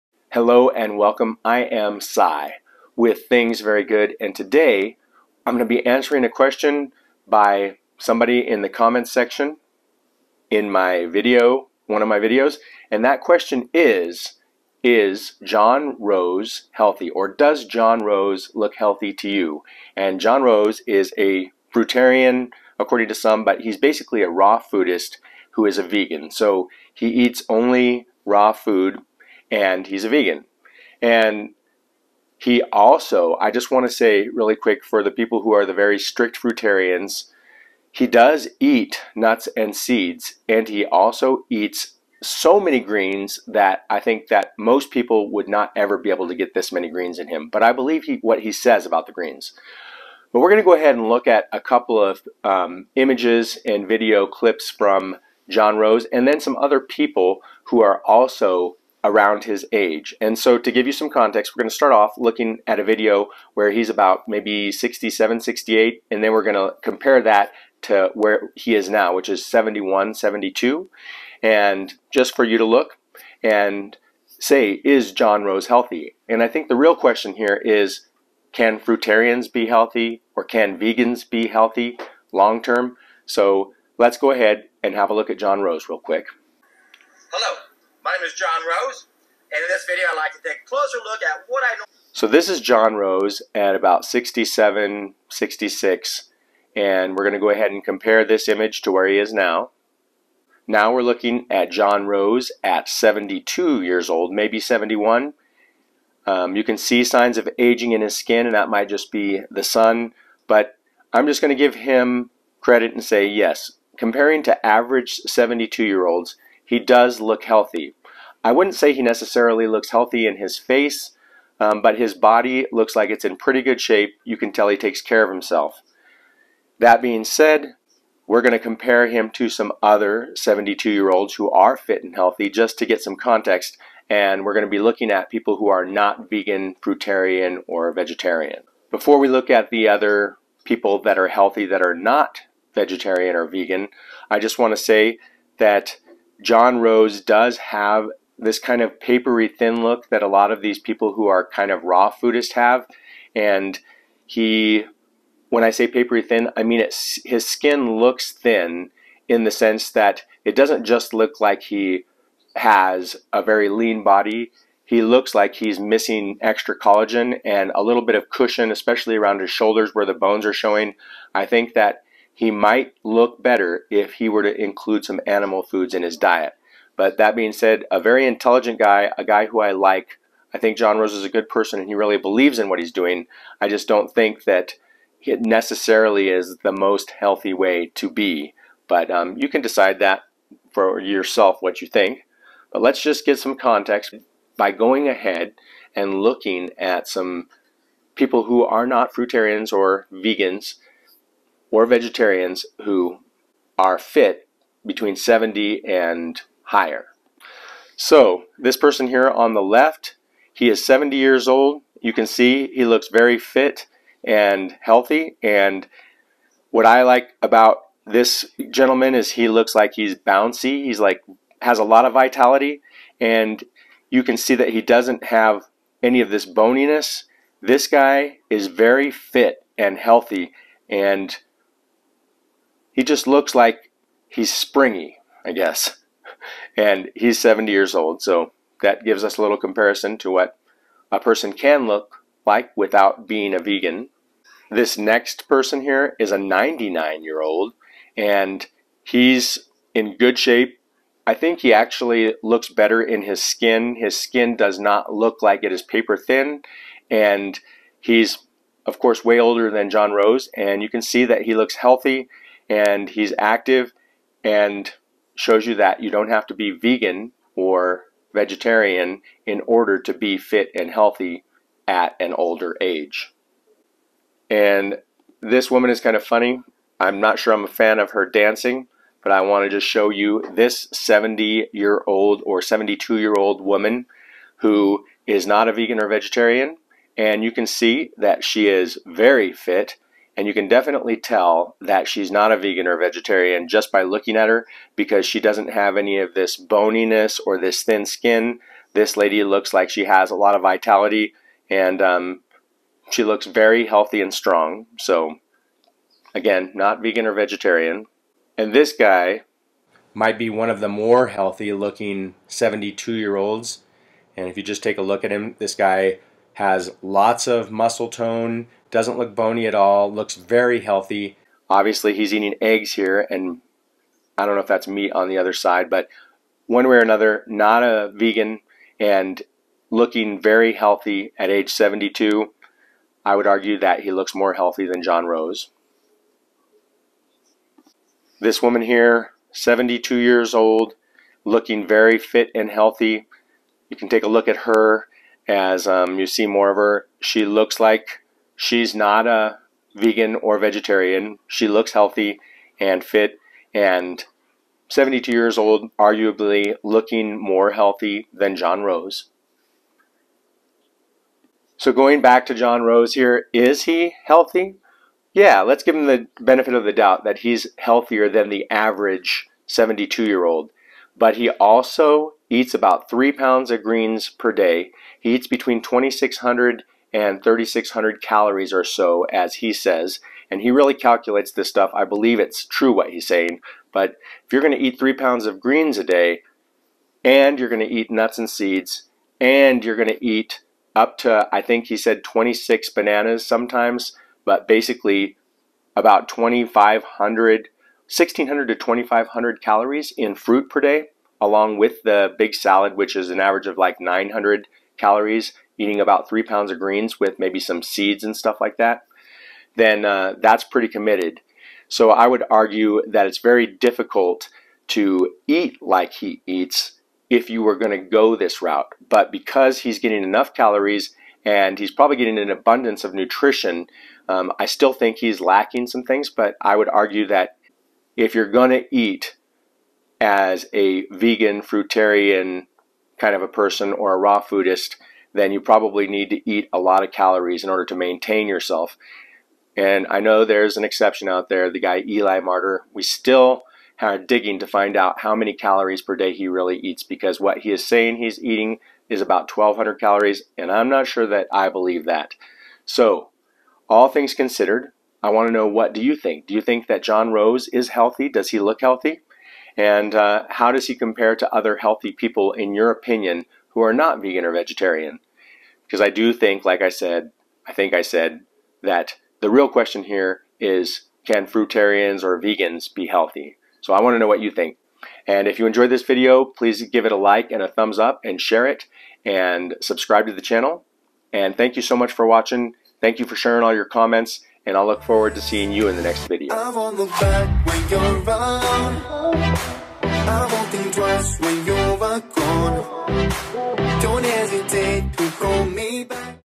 Does Fruitarian John Rose Look Healthy? ★ Sye Reviews
When it comes to Fruitarian, Vegan and Raw Food Diets I'm always looking at different people to see how things change over time as people follow these types of eating regimes.

I also follow people who have left these vegan diets and people that eat more animal based.

I'm fascinated with this subject and I am sharing my perspective with you.  I always appreciate your feedback so please comment and let me know what you think.

John Rose YouTube Channel

Scott Burnhard YouTube Channel

Localish YouTube Channel

Steve TV Show YouTube Channel

The Iconic Body YouTube Channel

GWHH19 YouTube Channel